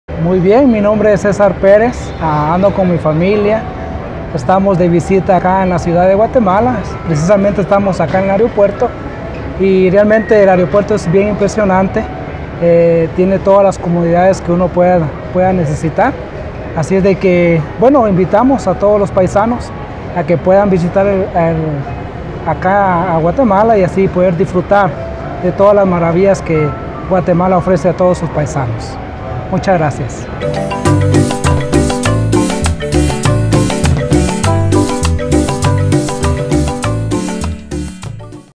20190902 La Voz del Viajero
👍✈ ¡Tu opinión, nuestro impulso a ser mejores cada día!

Nuestros usuarios manifiestan su satisfacción por el excelente servicio y los cambios impulsados por la administración de nuestro Director General, P.A. Francis Argueta Aguirre, en el Aeropuerto Internacional La Aurora 👏👏